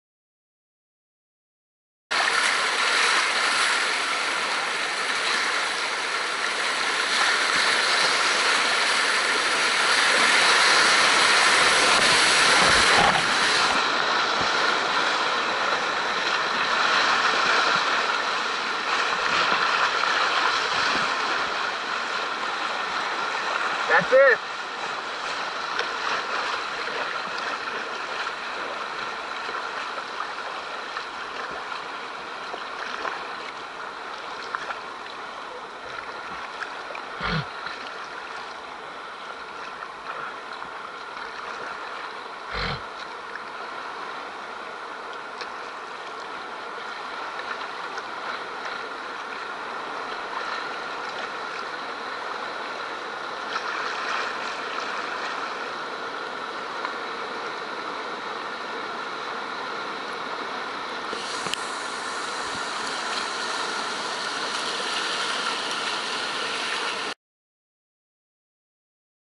Sony Gorge Test
A test of the new Sony AS100V. The video is from this past weekend on the South Fork of the American River in Coloma. Low flows but good times!